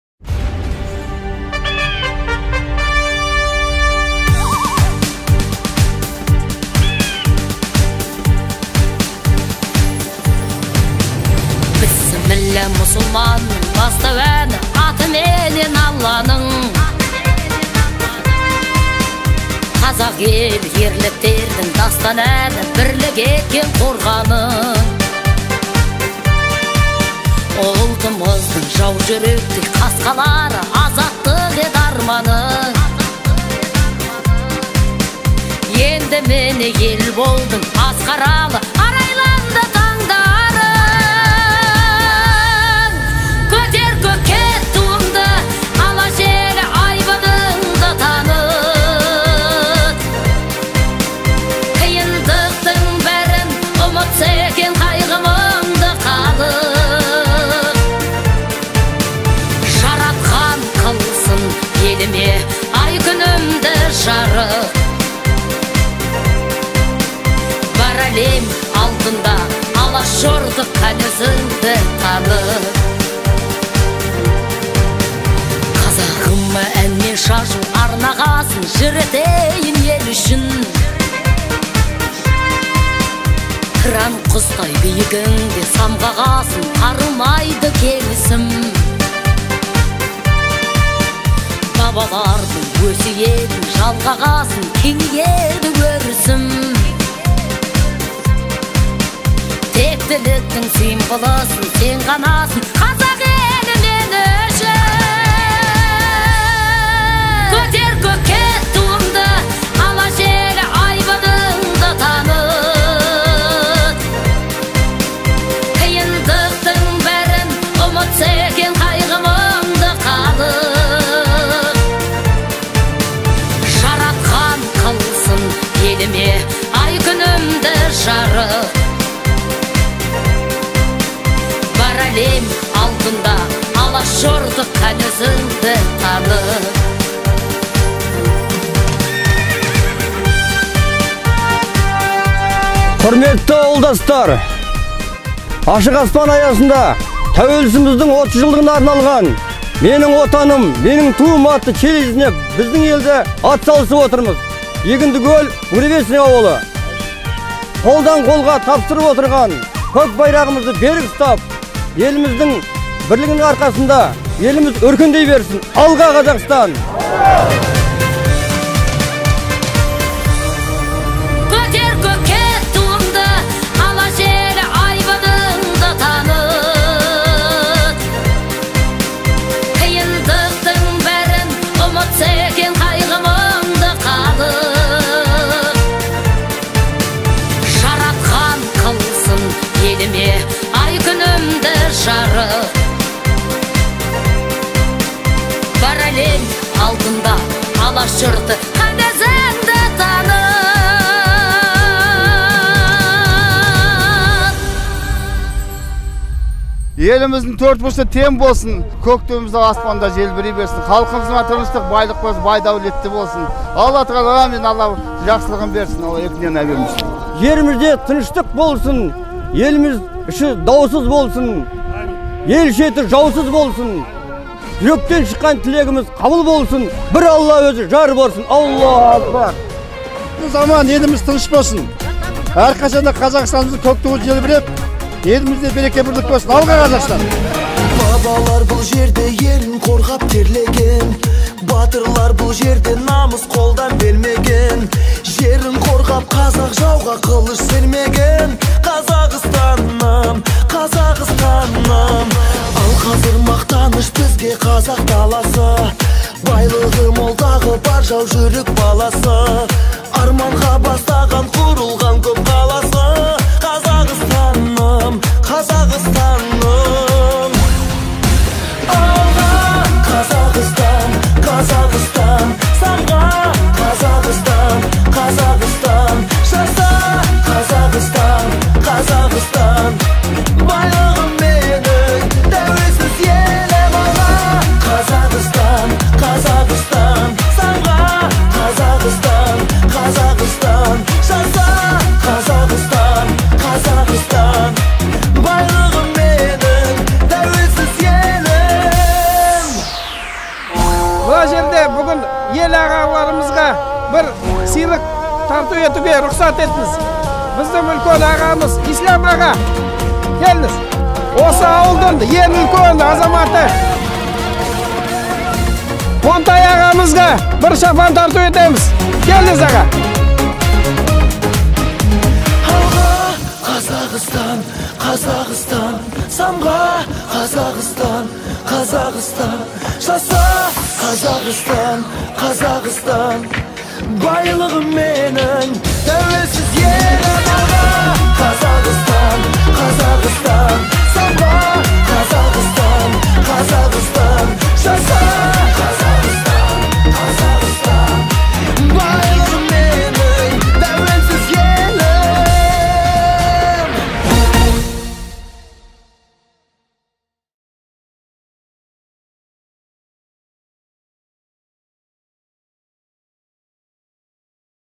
Буревестник селосы Егіндікөл Ауданы "Менің Туым Менің Отаным" челленджі
Челлендж МеніңТуымМеніңОтаным​ в селе Буревестник Егиндыкольского района